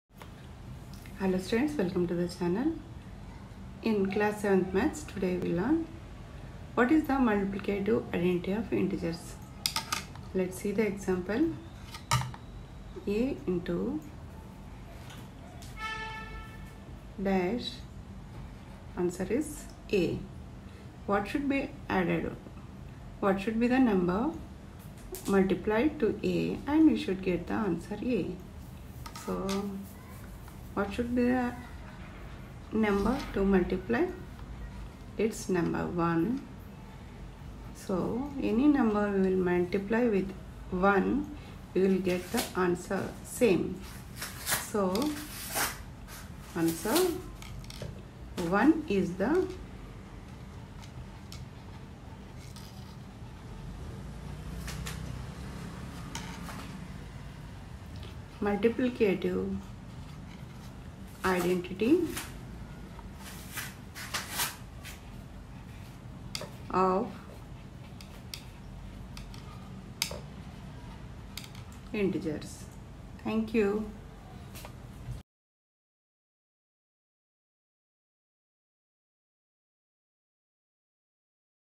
Maths: What is the Multiplicative Identity of Integers?
Maths: What is the Multiplicative Identity of Integers explained in detail with example.
Thanks